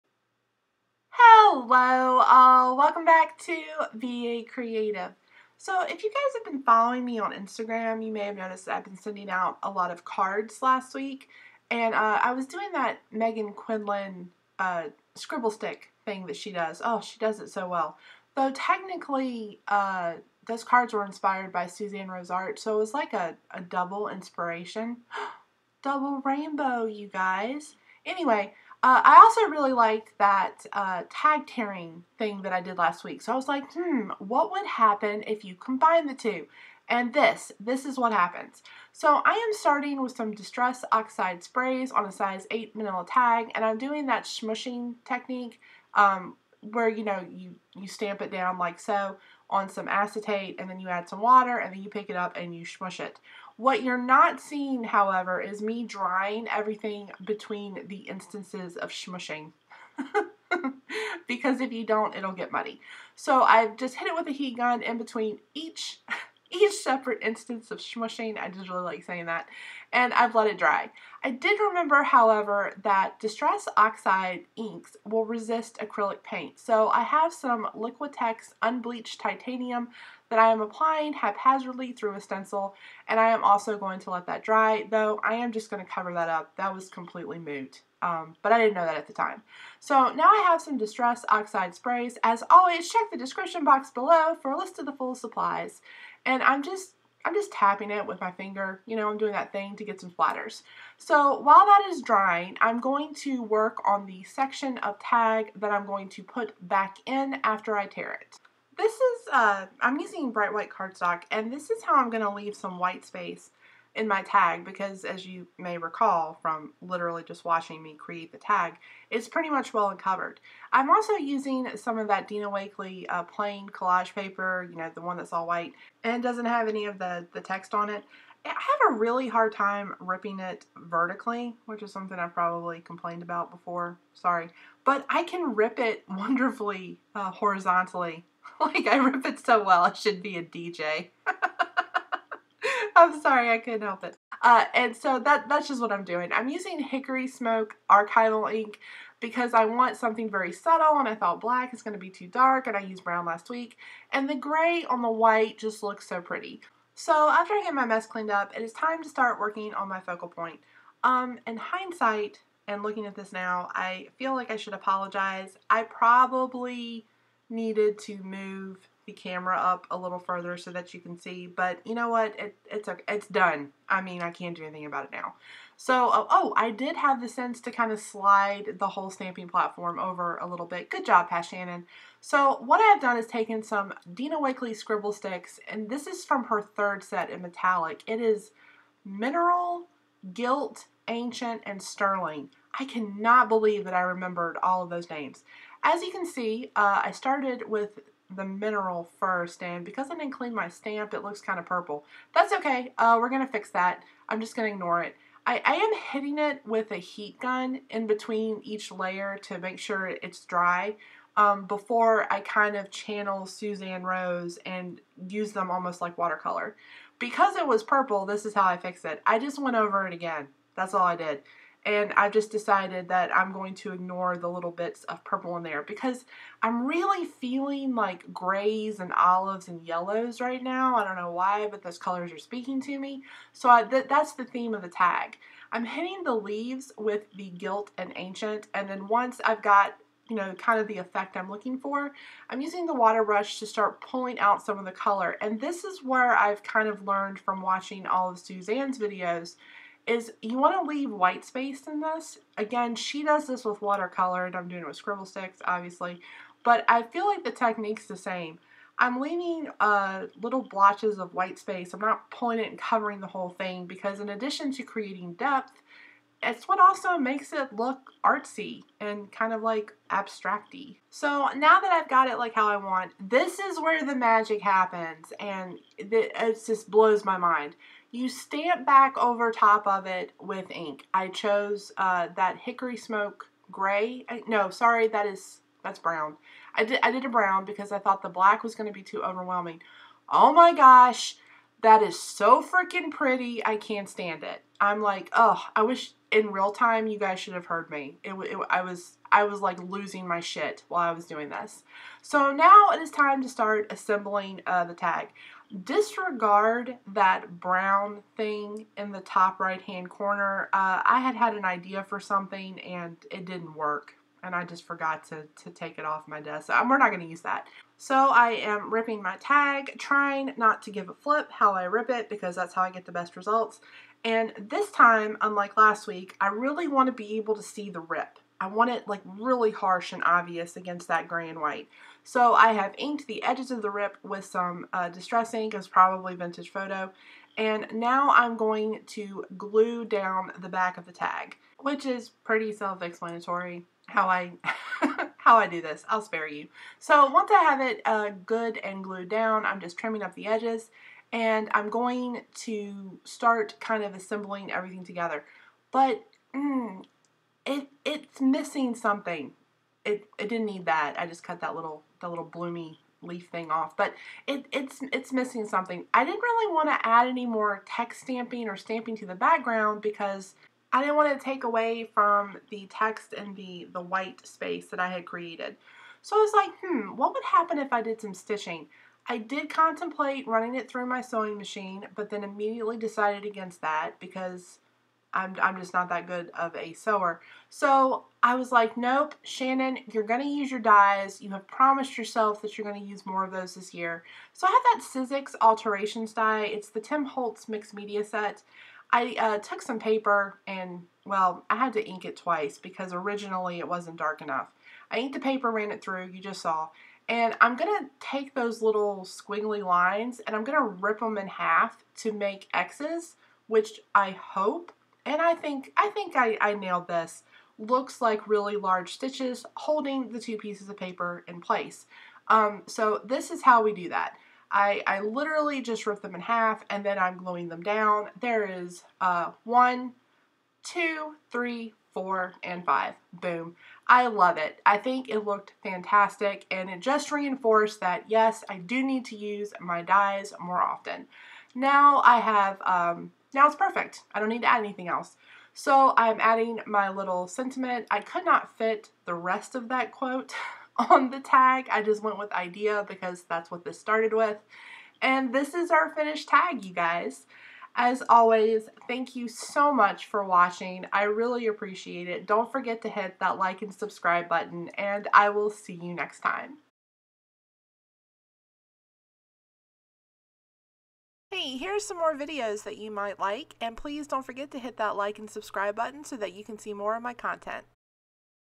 Mixed Media Art Tag with Scribble Sticks and Dina Wakley Stamps
Hello all! This week I've been on the Dina Wakley flower train and I don't think I"ll be getting off it for a while.  Last week I made some cards to send to friends after getting inspiration from Susanne Rose Art's beautiful watercolor tags and florals. Not having any Rubber Dance Stamps I thought self, you can do this with Dina Wakley's stamps.  Magic ya'll.

Supplies Used:
Dina Wakley Stamps: Always Flowers
Tim Holtz products: Distress Oxide Sprays- frayed burlap and weathered wood:
Dina Wakley Scribble Sticks- mineral, gilt, sterling and ancient
Sizzix Alterations Tim Holtz- Mixed Media